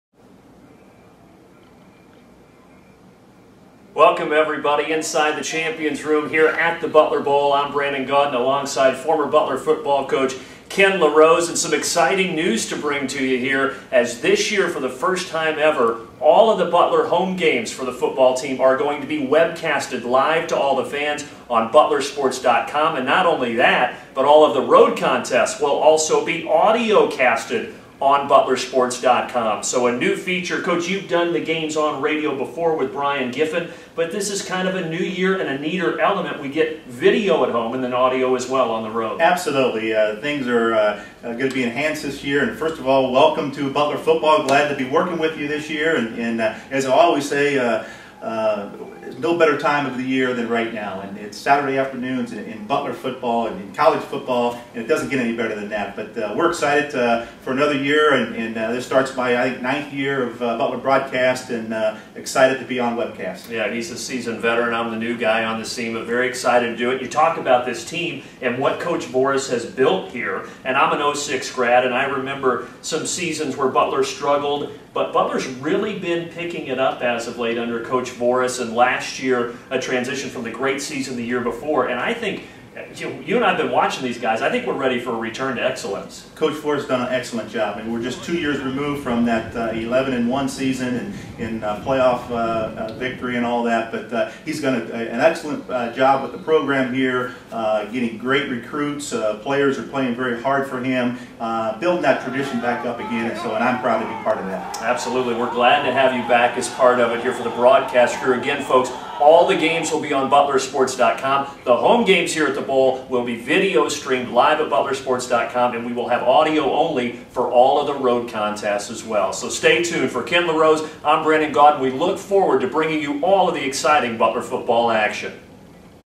FB Radio
Brandon Gaudin and Ken LaRose preview the 2012 season, as the Bulldogs' five home games will be streamed live on ButlerSports.com.